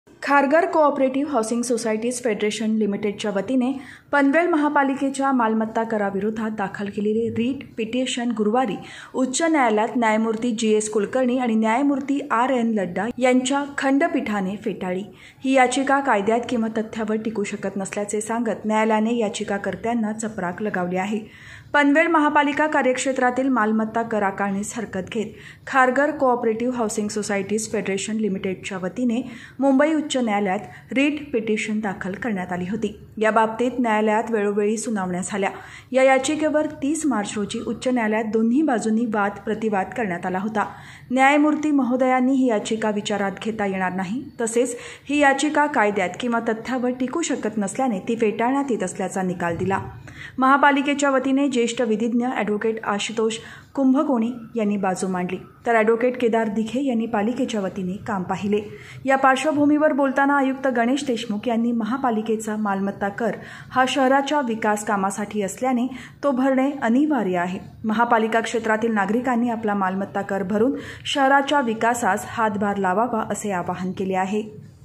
पनवेल मनपा मालमत्ता कराविरोधातील खारघर सोसायटीज फेडरेशनची याचिका हायकोर्टाने फेटाळली !
पनवेल पनवेल महापालिकेच्या मालमत्ता कराविरोधातील खारघर सोसायटीज फेडरेशनची याचिका हायकोर्टाने फेटाळली !malharnews